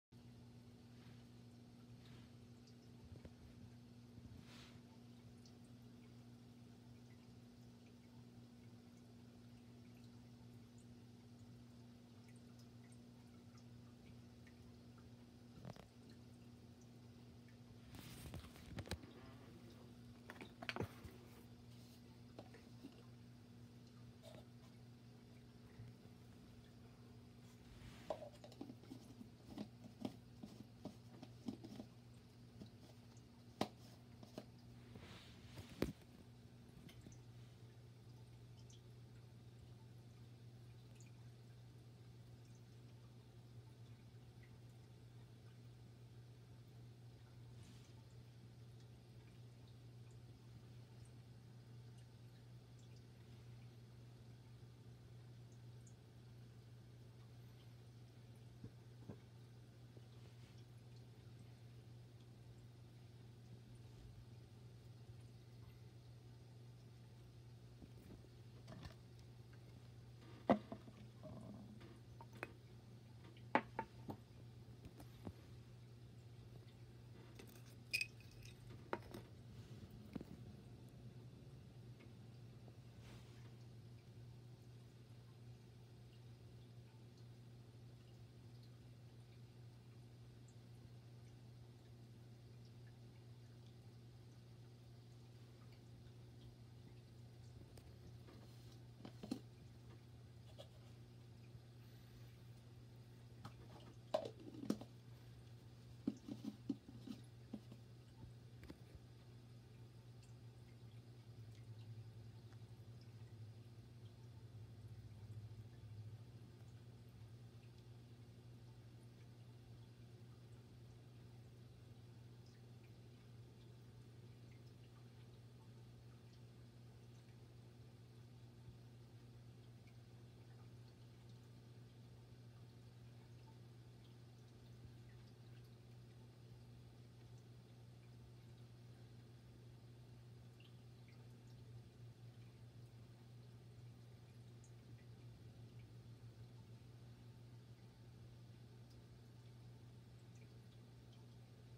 Ruby Red Dragonet Eating Flakes 22sec & 2:09
Does it at 22 secs and 2:09 for sure